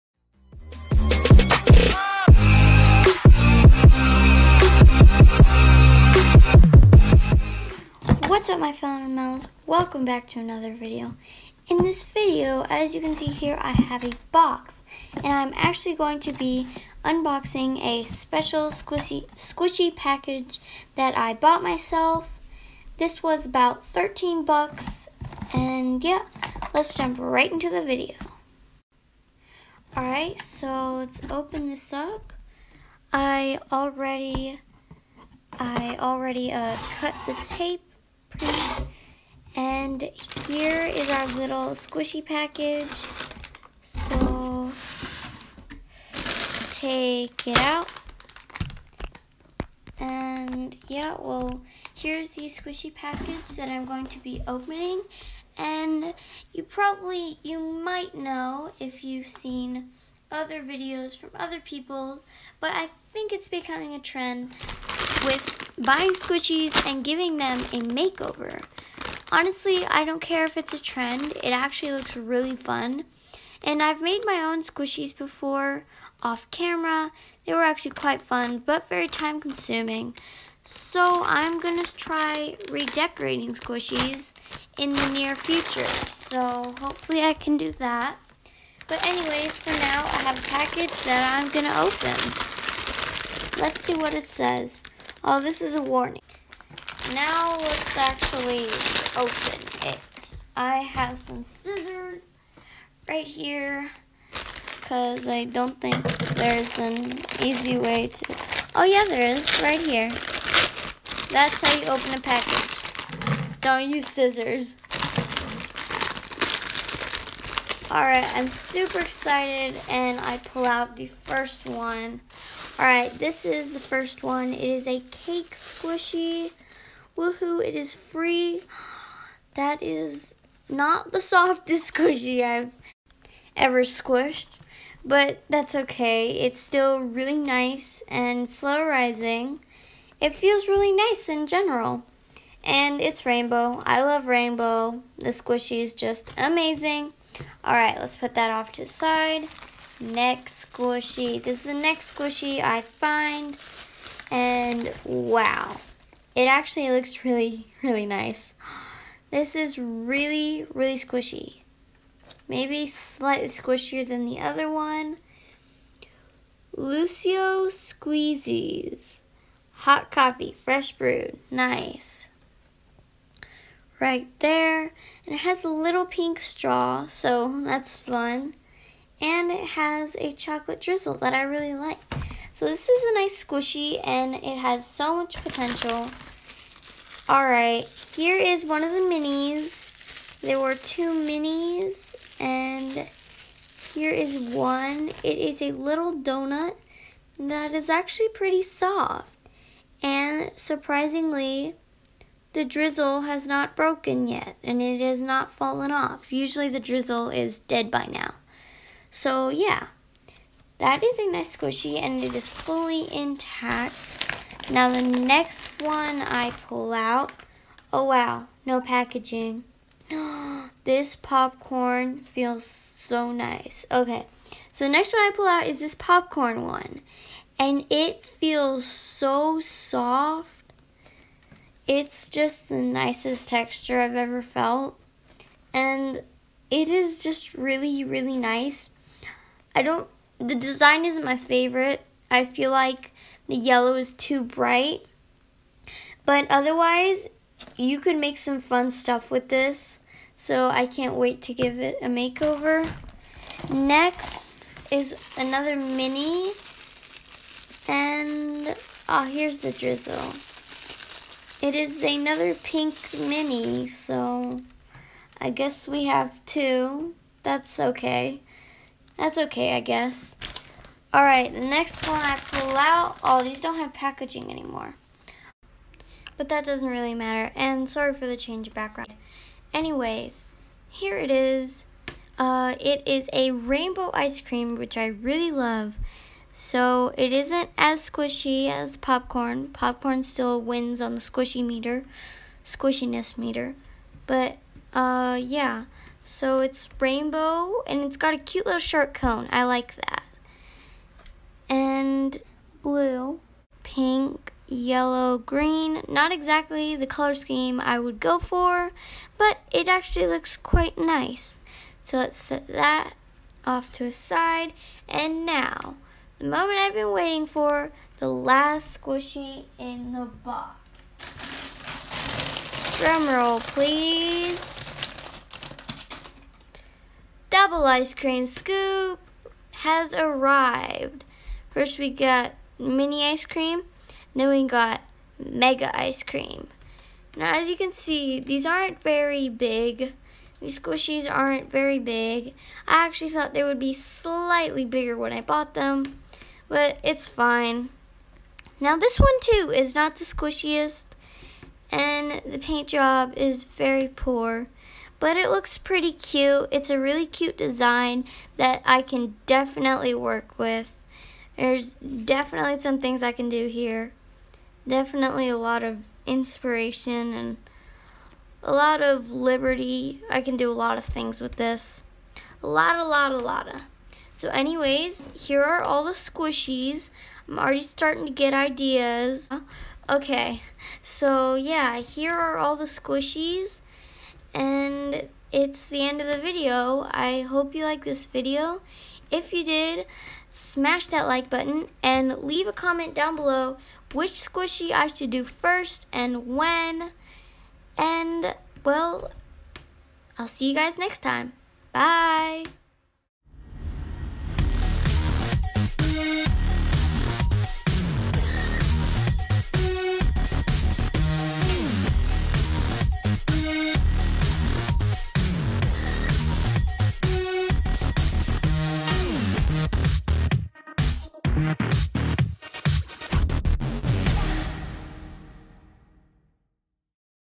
Unboxing SQUISHIES for a new series!!!!!!
In this video I’m going to be unboxing a squishy Amazon package, so I can later on make a new series with squishy makeovers!!! 📦 !